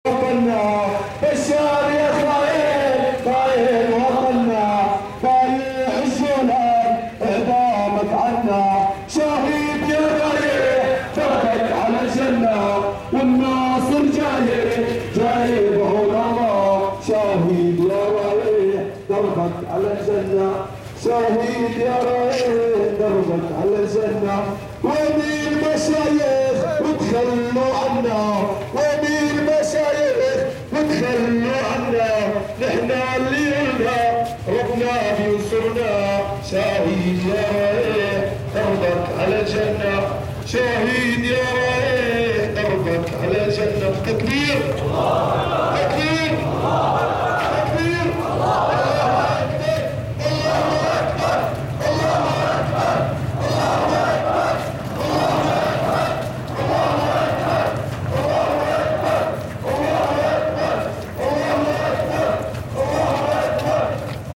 القلمون - قارة : تشييع الشهيد إياد البرشة17\12
القلمون - قارة : تشييع الشهيد إياد فؤاد البرشة 17\12\2012
تنسيقية قارة